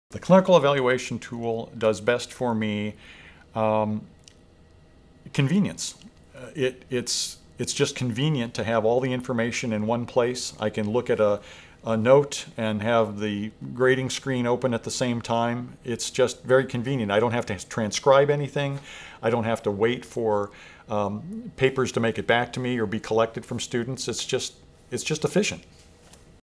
How Rubrics are Convenient for Faculty Evaluation of Students
The process of evaluating students in a clinical setting can be tedious and cumbersome - here's how Marian University College of Osteopathic Medicine has made the process efficient and convenient for faculty and students.